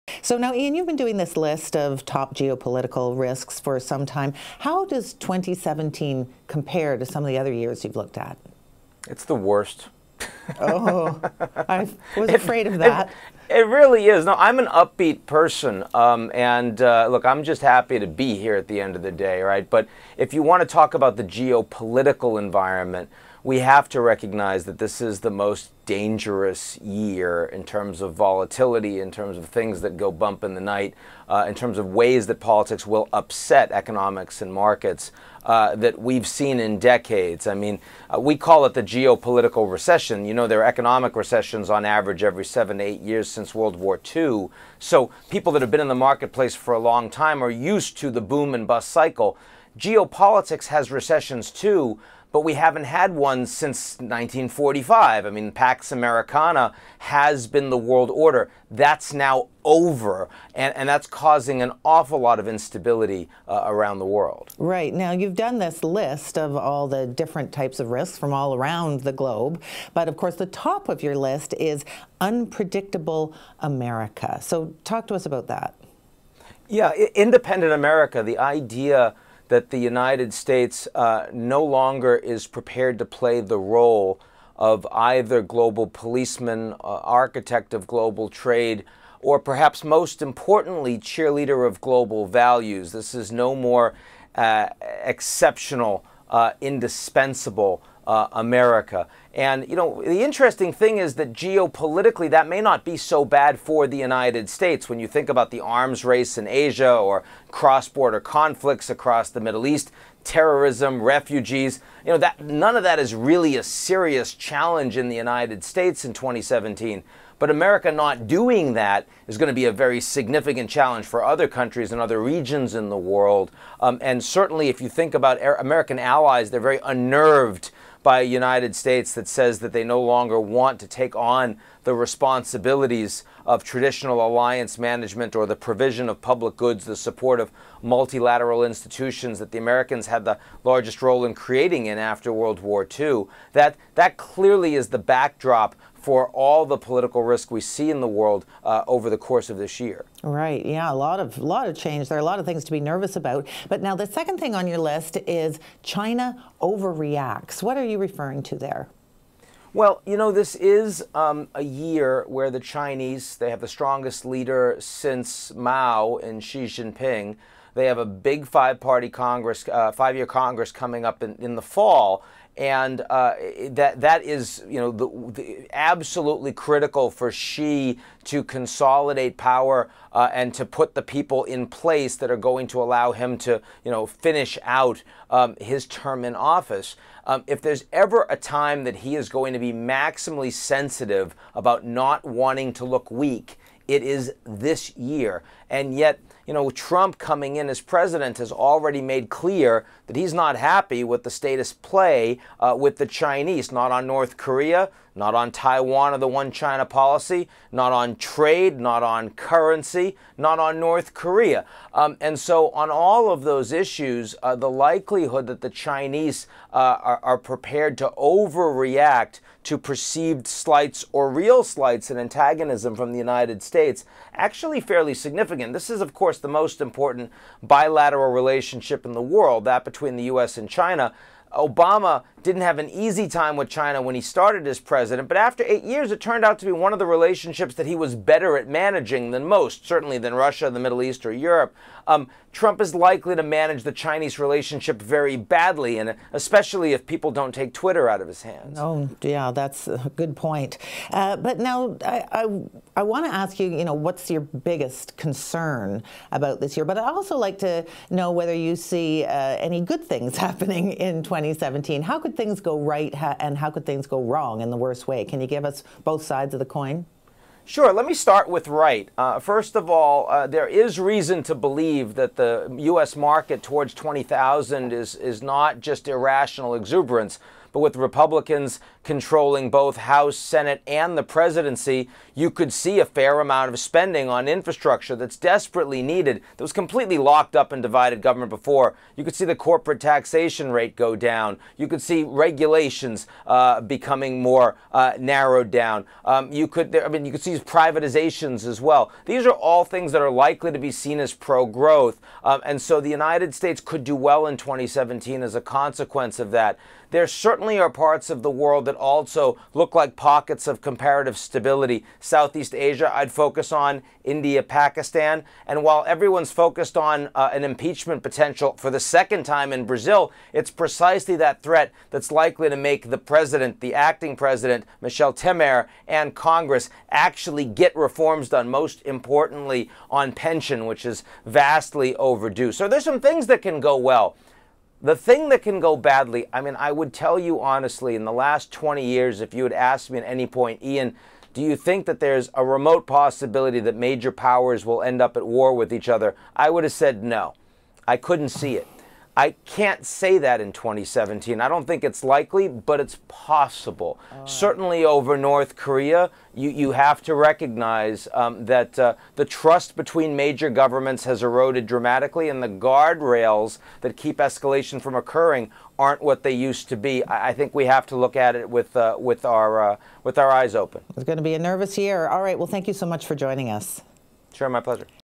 Top geopolitical risks for 2017
Ian Bremmer, founder, Eurasia Group, on where the hot spots will be in 2017. To read Brian Stewat's take